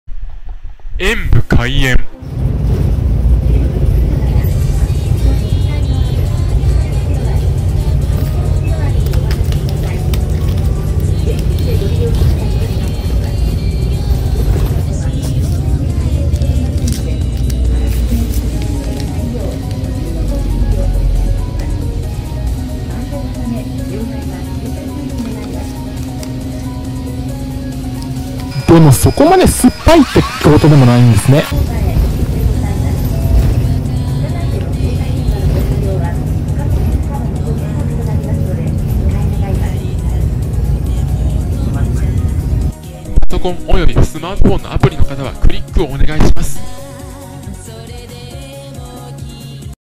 【サイレント】🍫明治ザ・チョコレートベルベットミルクをただ食べるだけ。
明治　ザ・チョコレートベルベットミルクを食べてみました。
ベルベットって何なんだろう？昔そんな名前のゲームキャラクター居たような気が……
贈り物は此方まで↓
〒153-0042